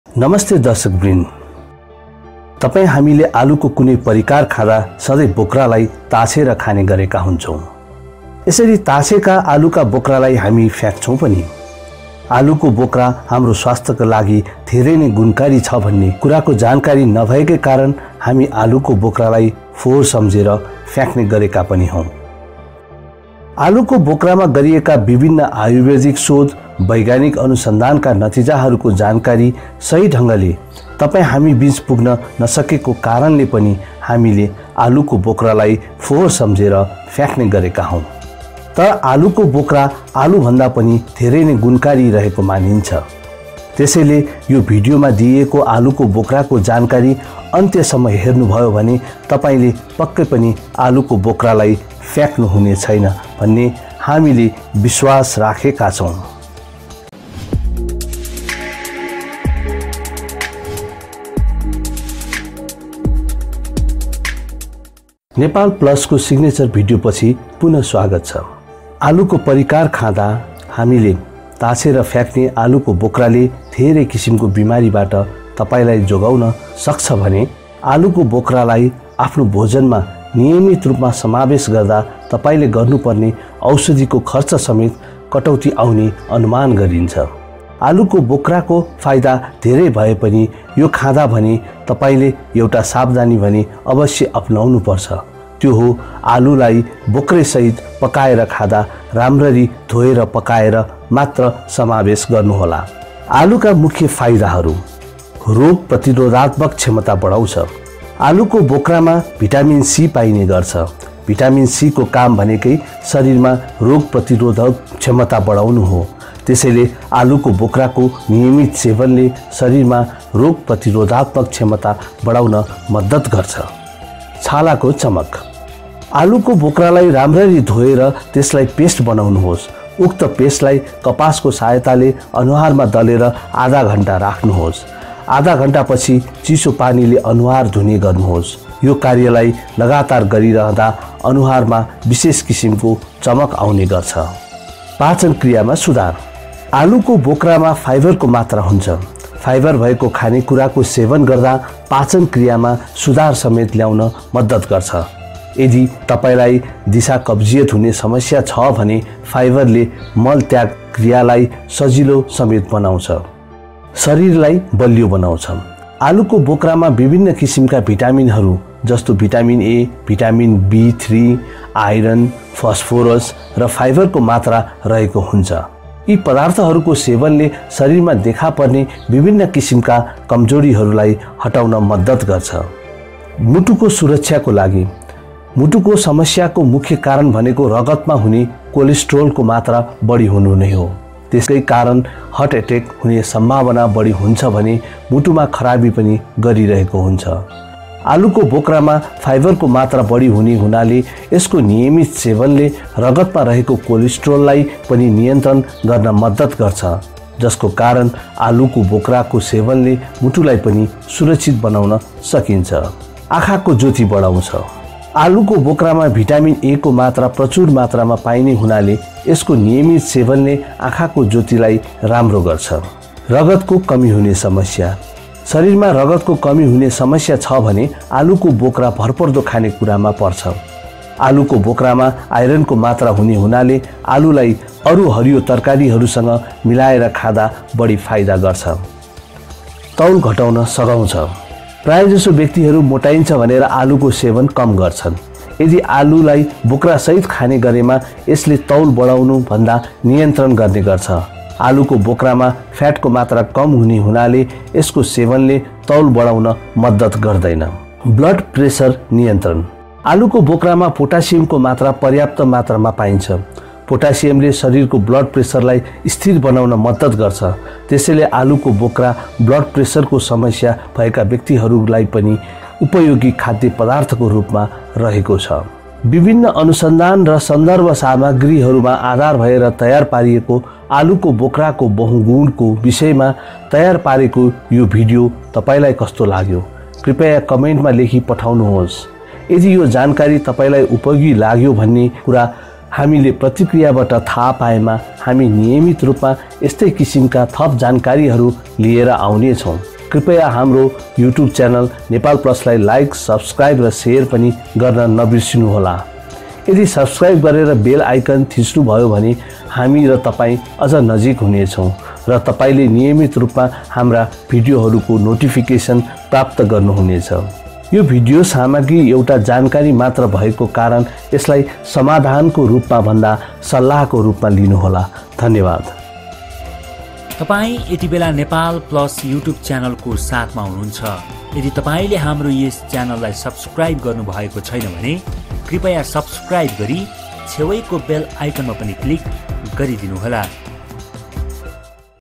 आलुको बोक्राका अनौठा फाइदाहरू II Benifit of Potato Skin
तपाई हामी आलुको बोक्रा फ्याँकेर मात्र खाने गरेका हुन्छौं । आलुको बोक्रा हाम्रो स्वास्थका लागि धेरै नै गुणकारी मानिन्छ । यो कुराको जानकारी नभएकै कारण हामी आलुको बोक्रालाई फोहोर संझेर फ्याक्ने गरेका पनि हौं ।
आलुको बोक्रामा गरिएका विभिन्न आयुर्वेदिक सोध, बैज्ञानिक अनुसन्धानका नतिजाहरुलाई हेर्ने हो भने आलुको बोक्रा आलु भन्दा पनि धेरै नै गुणकारी मानिन्छ । त्यसैले यो भिडियोमा आलुको बोक्राको जानकारी बिस्तृतमा दिने प्रयास गरिएको छ ।